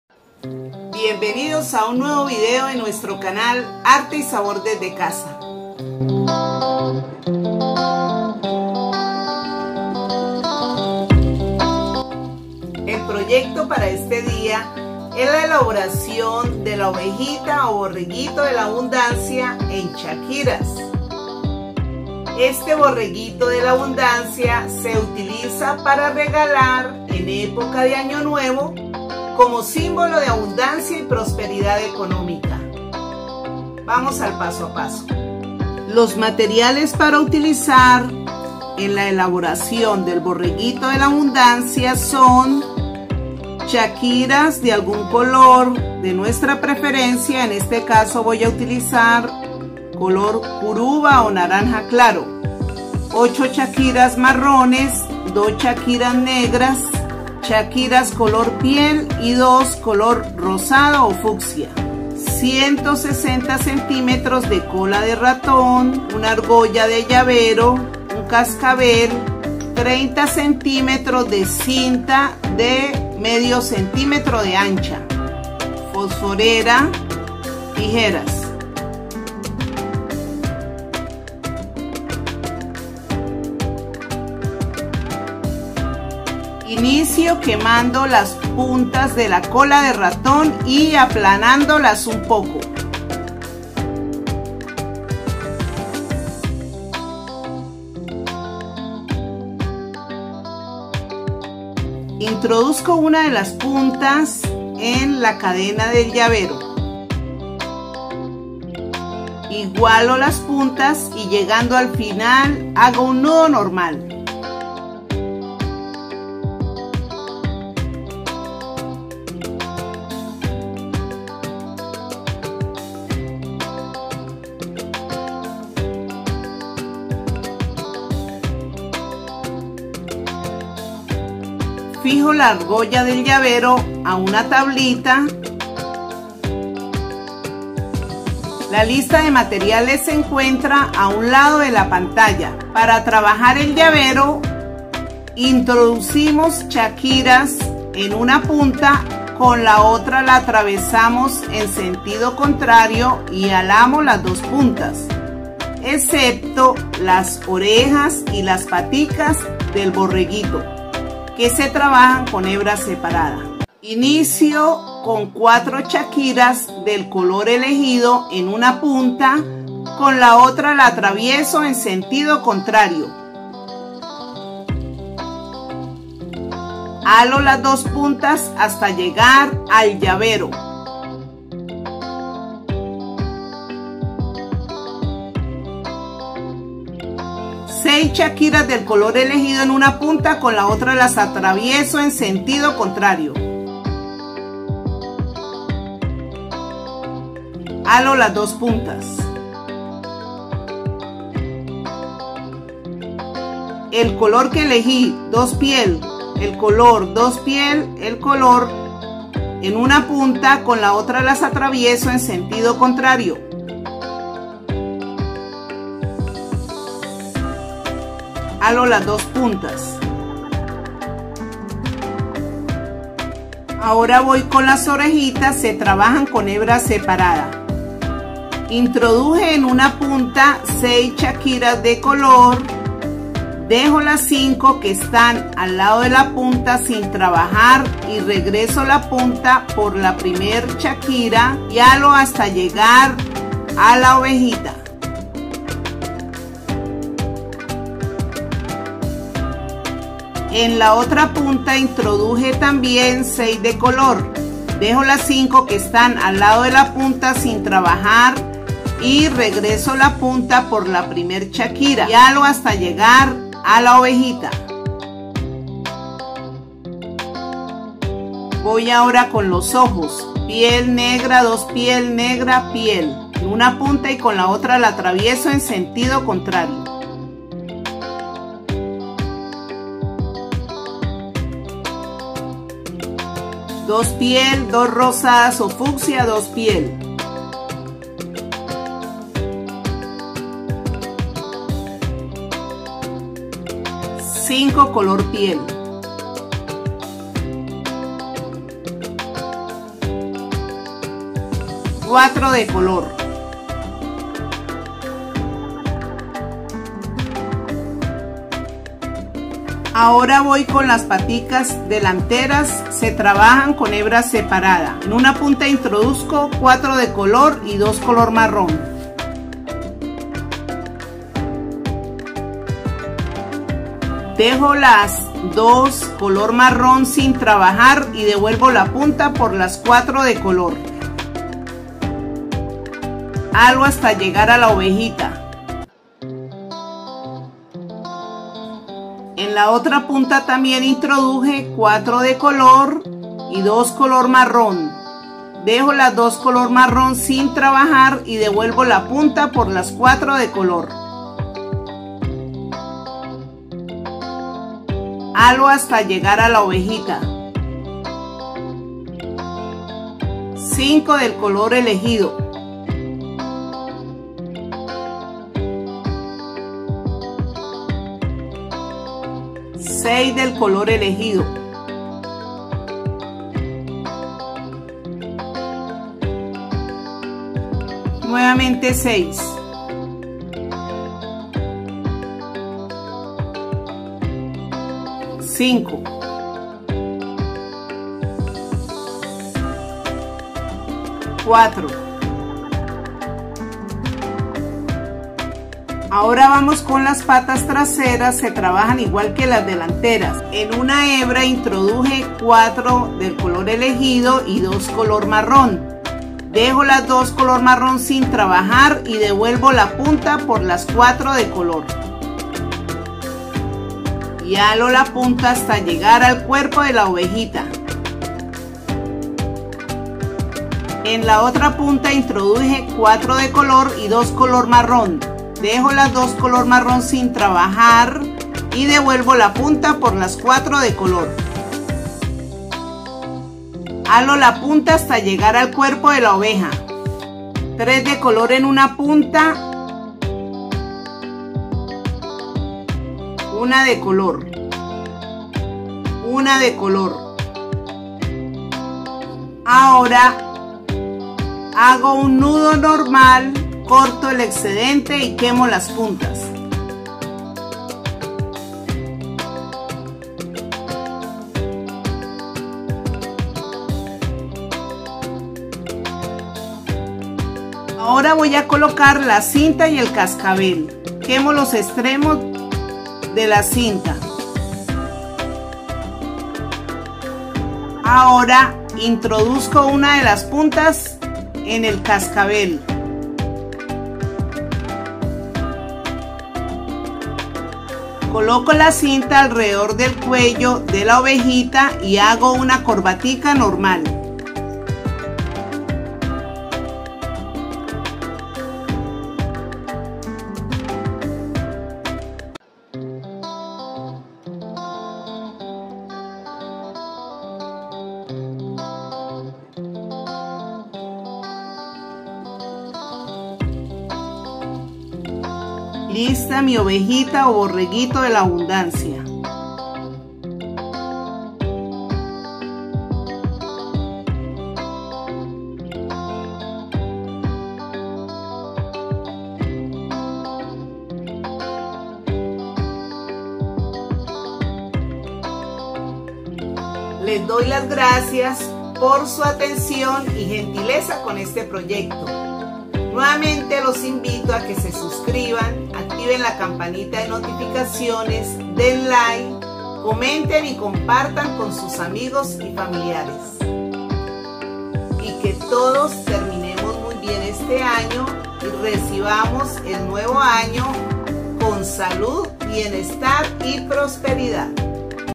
Oveja o borrego de la abundancia en chaquiras o cuentas kandi, paso a paso!♥
Con amor y cariño♡